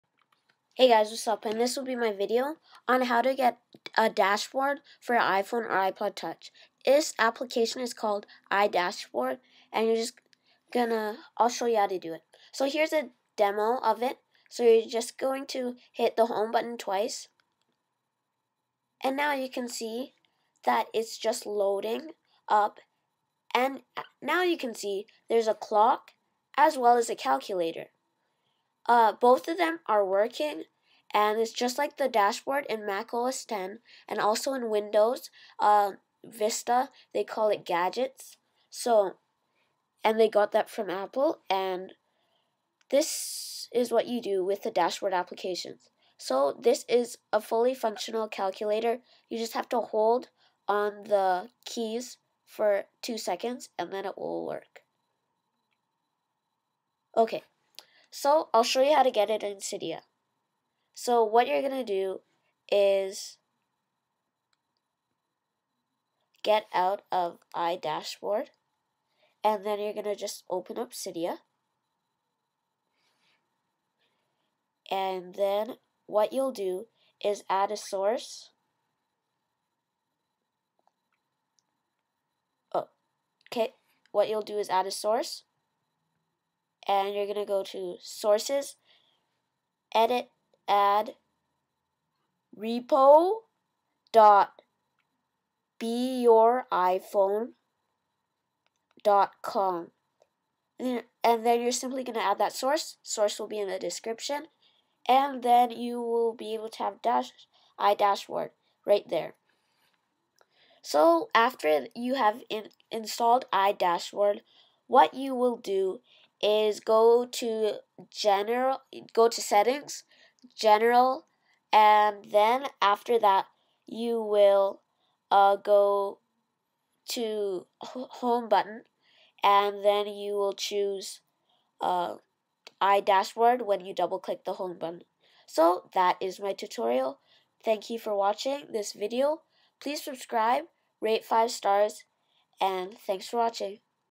How to Get Dashboard for iPhone / iPod Touch (iDashboard)
2. Go to winterboard and enable DashBoard
3. Go to Setting-General-Home-Dash Board
4. =D double click the homebutton to enable it